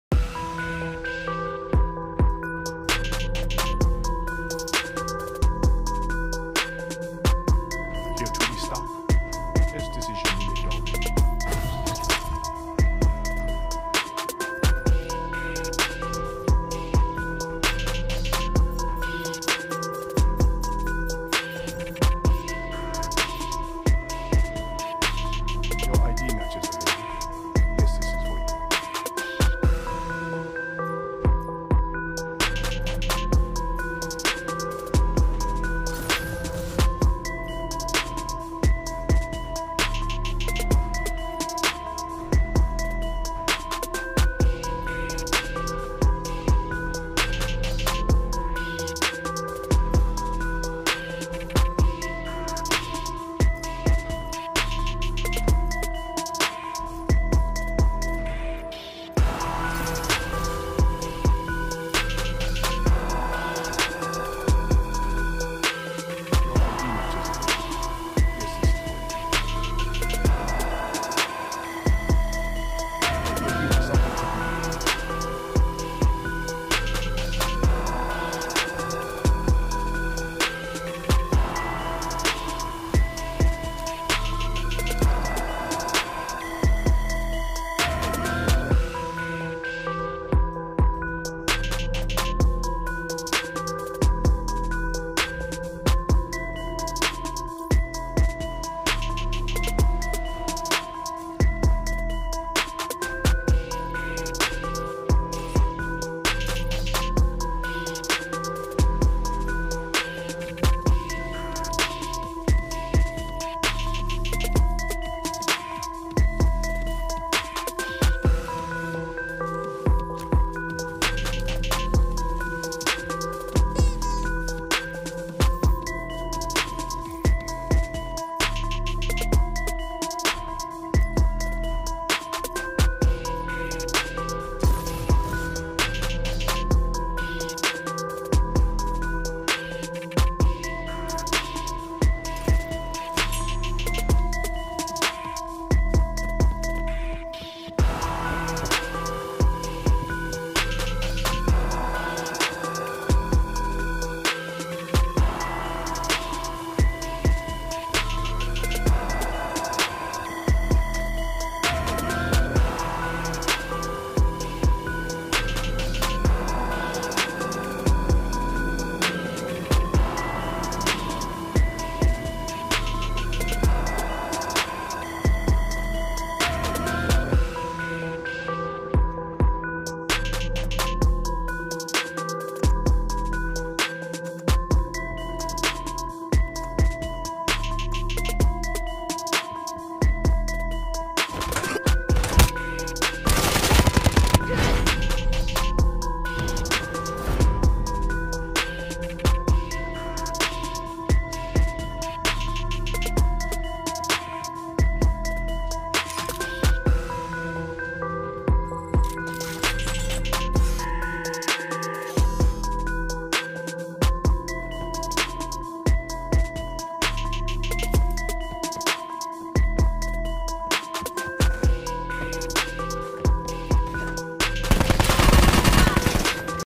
working on special order dupe and this happened dont do this . stuck in door
B-MAN got stuck in a door while doing test on a special order dupe
dupe not finished im working on the dupe